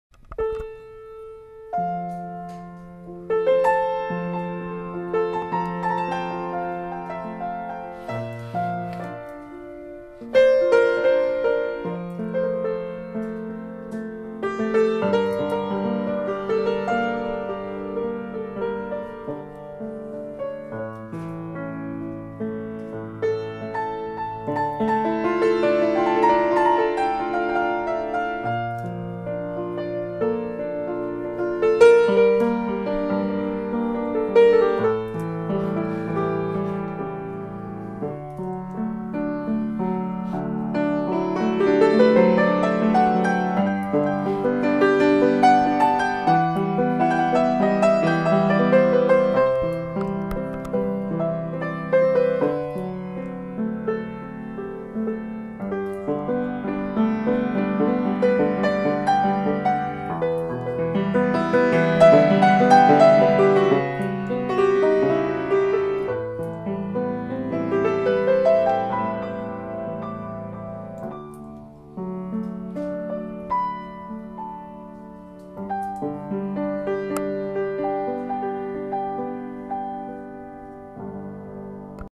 Petite - baby grand piano demo by
4'9  baby  baby grand piano  with  an adequate  sound  for  a small  salon.,
Price is super low for this brand new piano , go to order one right away!